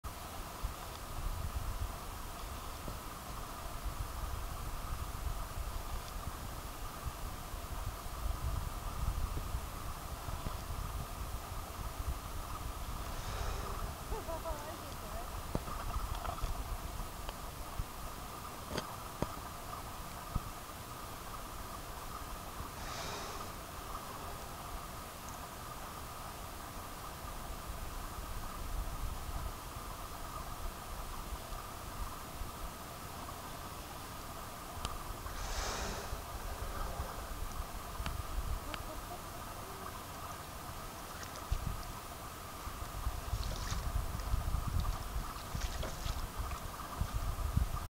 Gray Whale visit while rowing
Gray Whale in Totten Inlet -- Olympia Washington.